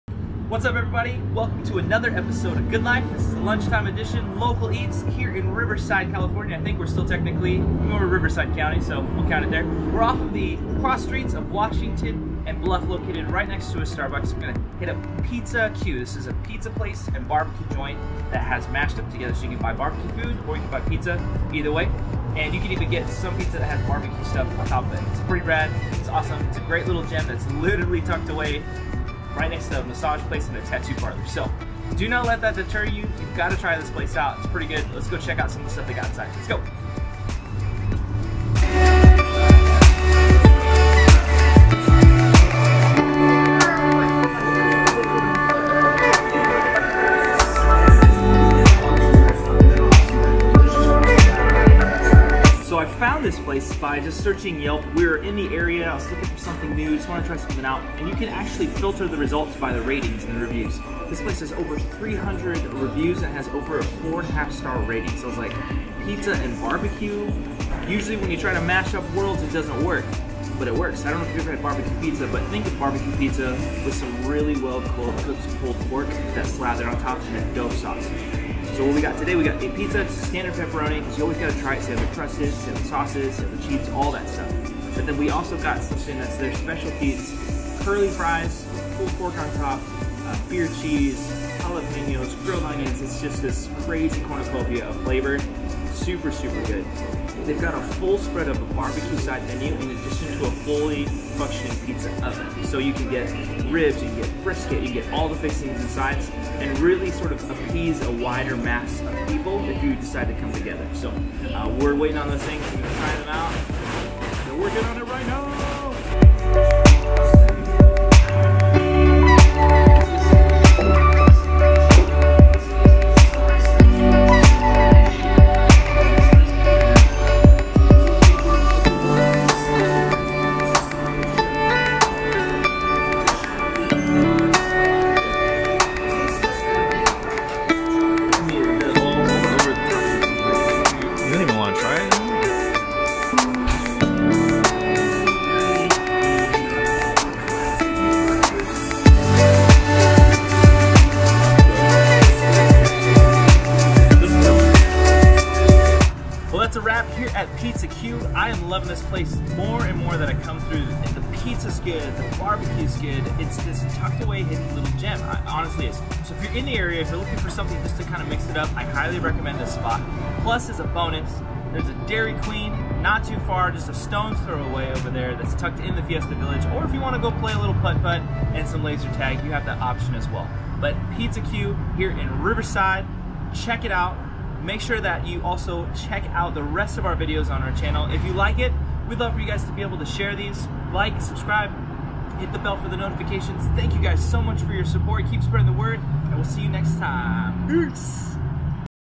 Good Life | Lunch Break: Pizza-Q
This week we take a quick lunch break to check out another hidden gem on the edge of Riverside and Colton. Pizza-Q is a pizza and BBQ joint that seems to know how to do both pretty well. Join us for a quick stop to see what we thought!